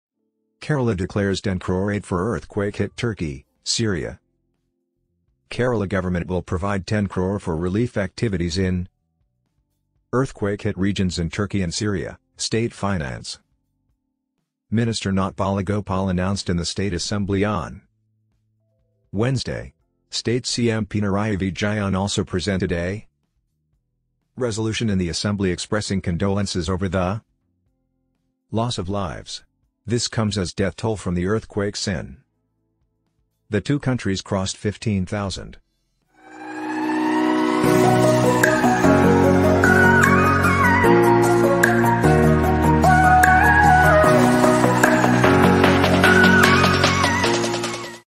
Kerala declares ₹10 crore aid for earthquake-hit Turkey, Syria
Kerala government will provide ₹10 crore for relief activities in earthquake-hit regions in Turkey and Syria, state Finance Minister KN Balagopal announced in the state Assembly on Wednesday. State CM Pinarayi Vijayan also presented a resolution in the Assembly expressing condolences over the loss of lives. This comes as death toll from the earthquakes in the two countries crossed 15,000.